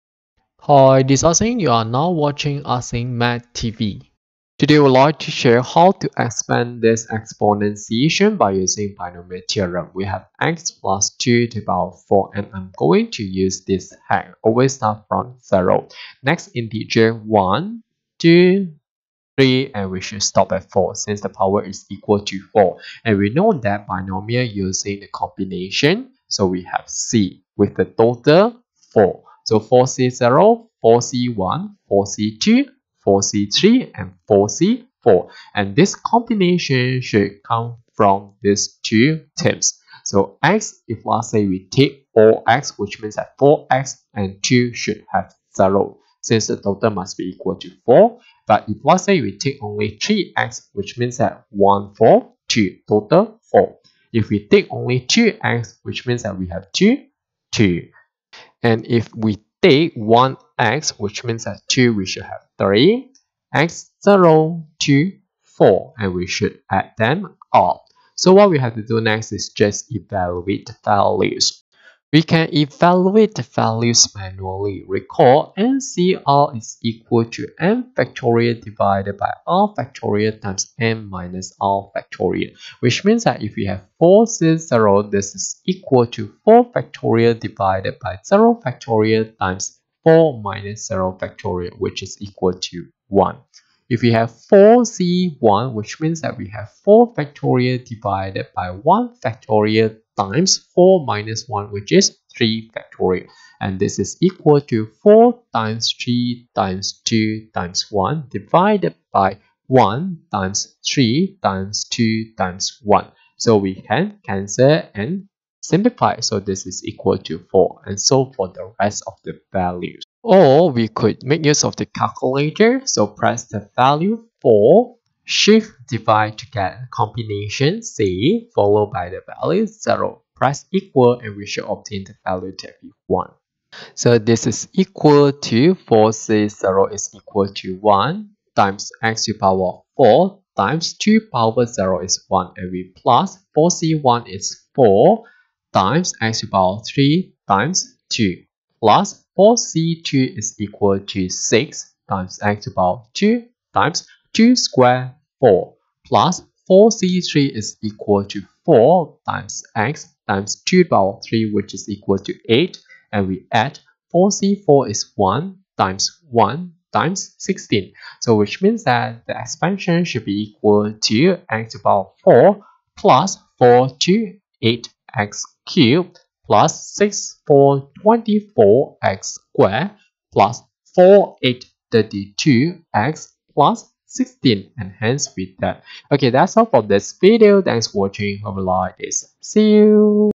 Use binomial theorem to expand (x+2)^4
A simpler way of applying the Binomial Theorem.
Expand (x+2)^4 by using the Binomial Theorem.